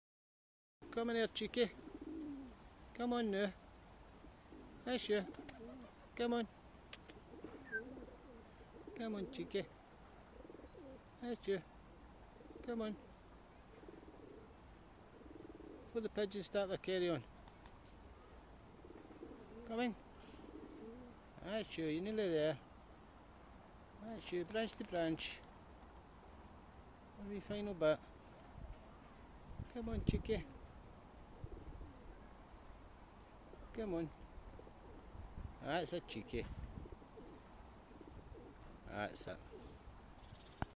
Blue tit Lands Can't take Nut 29 07 2020
Hand feeding Blue Tit in Kelvingrove park, Glasgow.  July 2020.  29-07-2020.